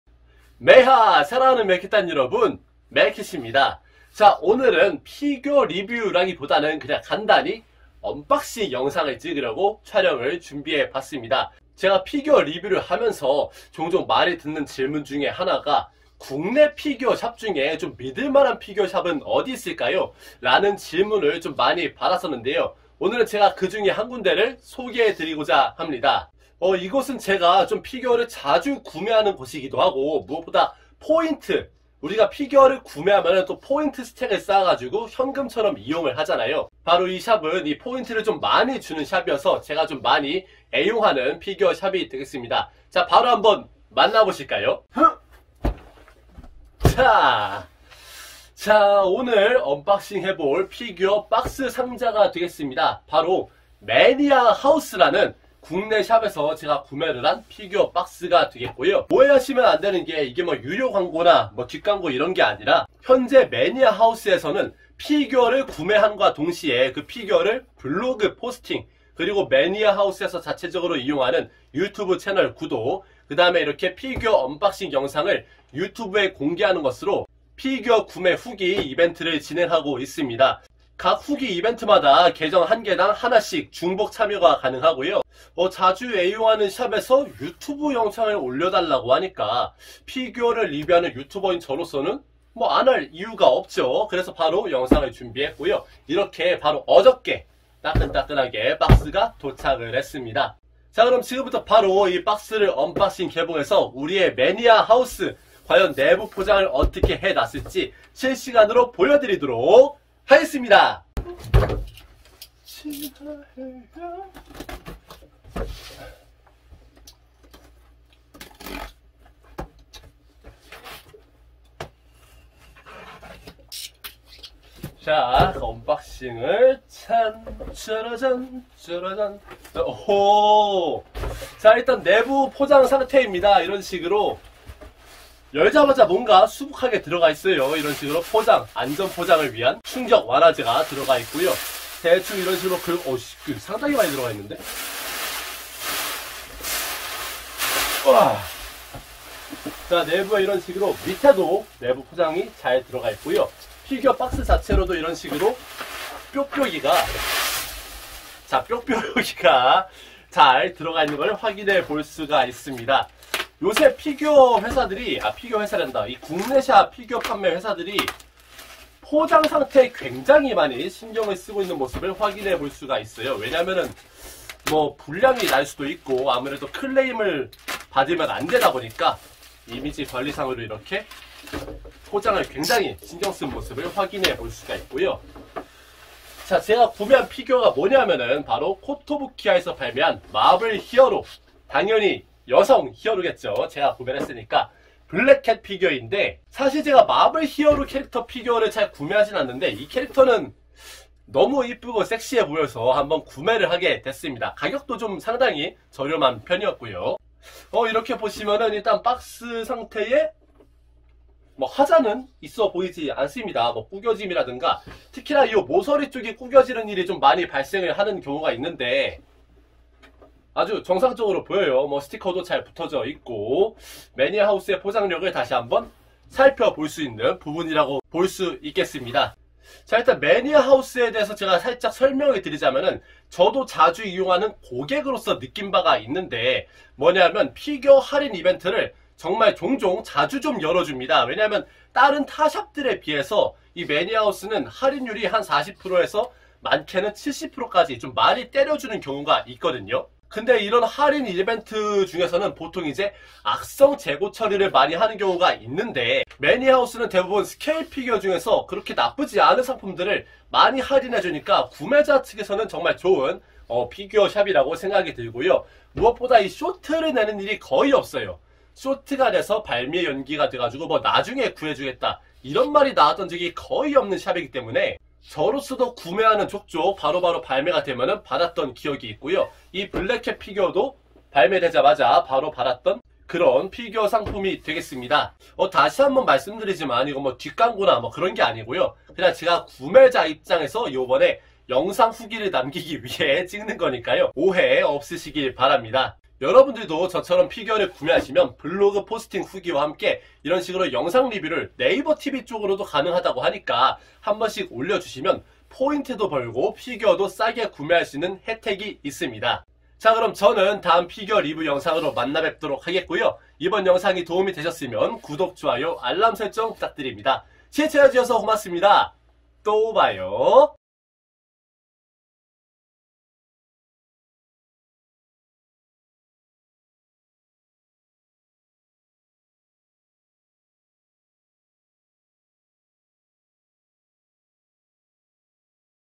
(국내 피규어샵 추천) 매니아 하우스 피규어 구매 후기!! 이게 피규어샵이제! 아직도 모르는 흑우 없제?? 매니아 하우스 구매후기 이벤트 영상
본 영상은 매니아 하우스에서 현재 진행하고 있는 영상 후기 이벤트 참가용 리뷰 입니다. 해당 샵에 대한 광고나 금전적인 보상을 받지 않았음을 알립니다.

 미소녀 피규어 리뷰 채널 : 저세상 피규어